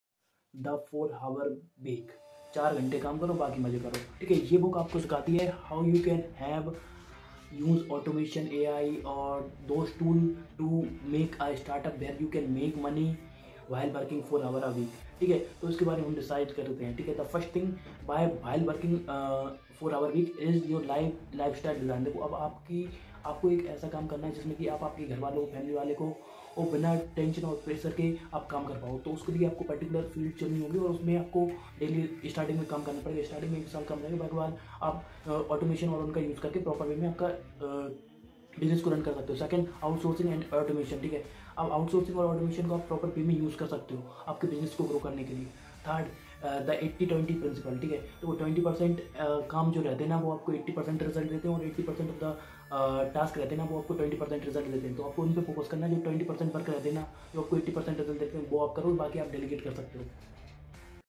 the four hour workweek book sumarries 3 key takeaways explained in hindi
your queries solved in this video in details